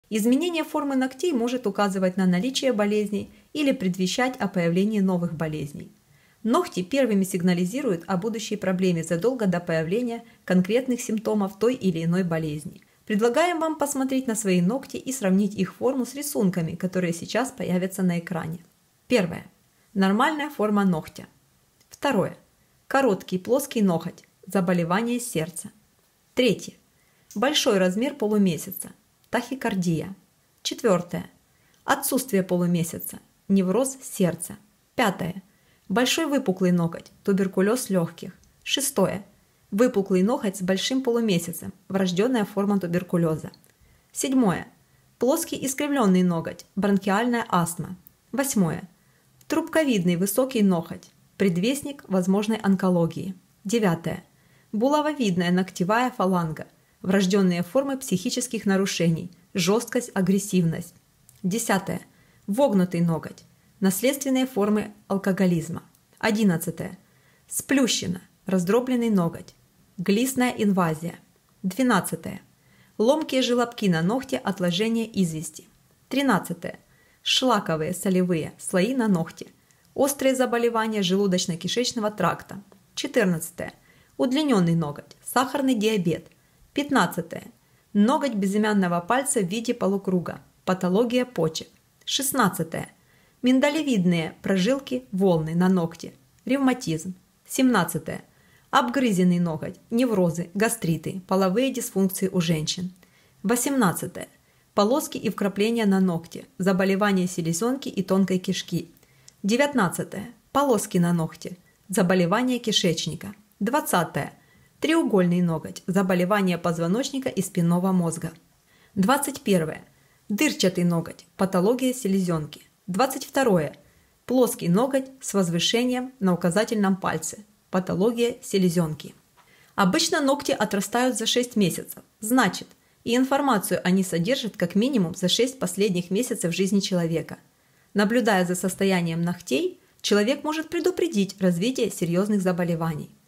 Как ногти могут рассказать о вашем здоровье: диагностика
Вы когда-нибудь задумывались, что ваши ногти могут быть индикатором здоровья? Узнайте, как по состоянию ногтей можно определить различные заболевания и нарушения в организме.